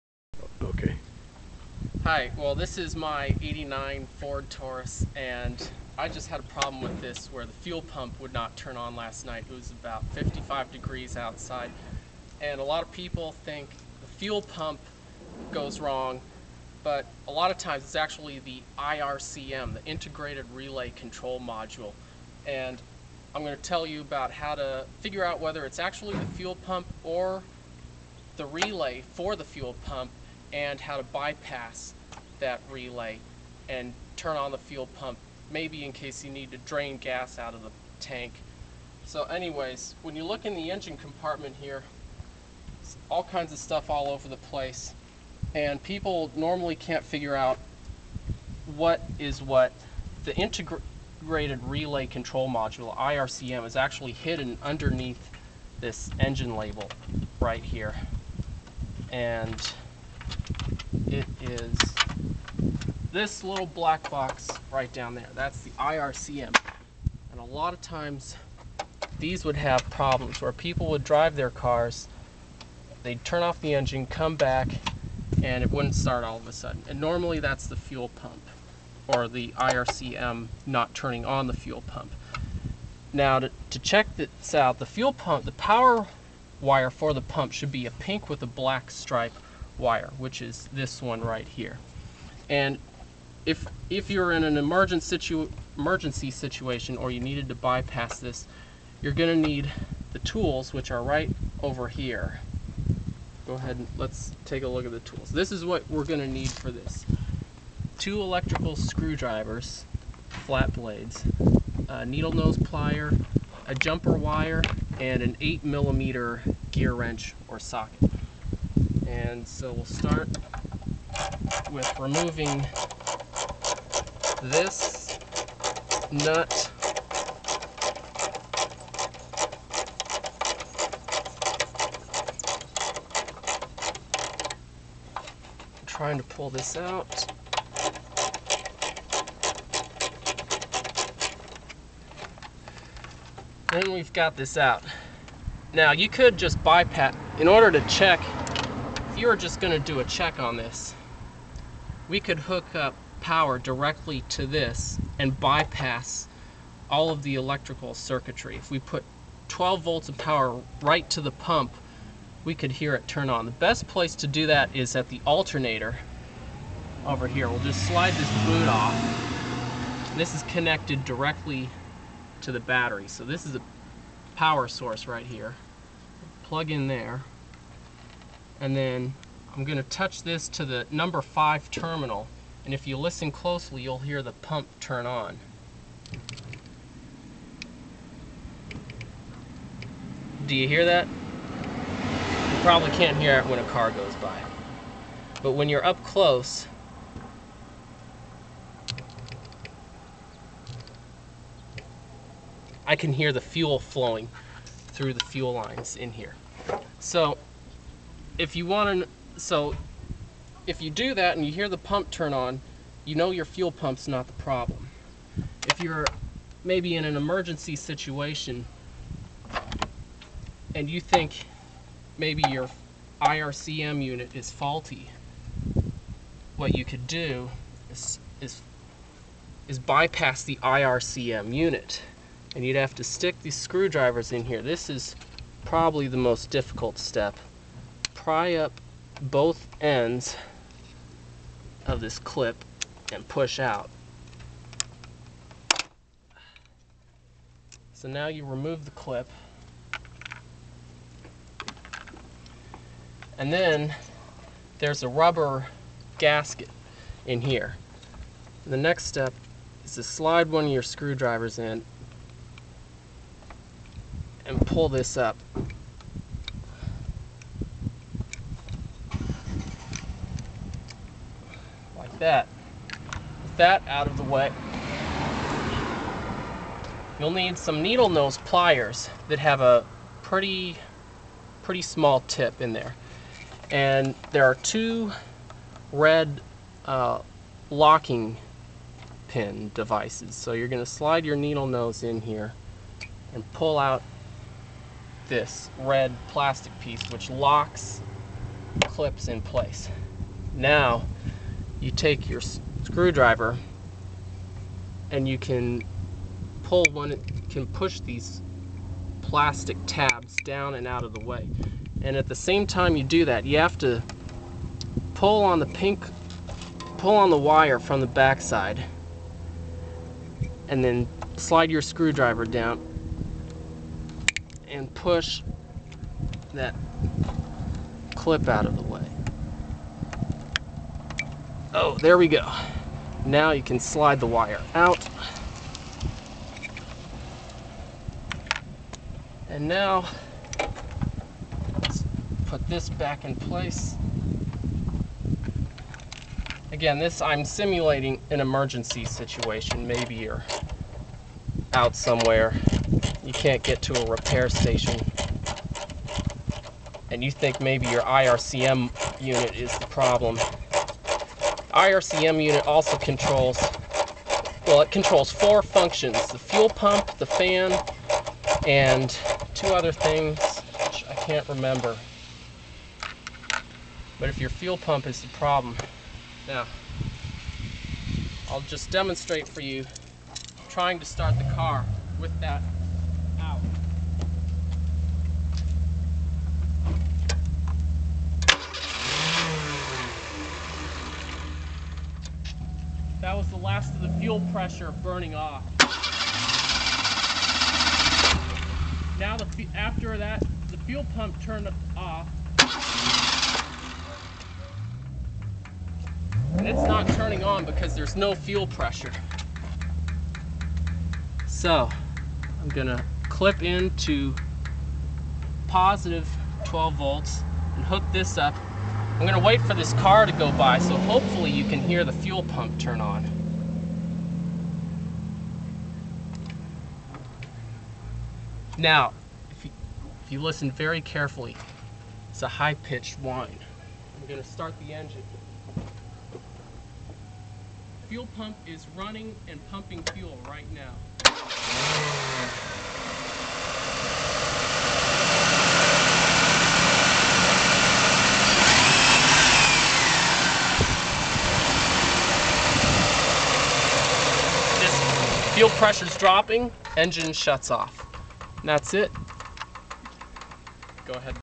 How to check the fuel pump
You can also check for fuel pressure by pressing in on the Shrader valve on the fuel rail.  If you put 12 volts directly to the pump and it won't turn on, then you either have a bad fuel pump or a bad ground circuit.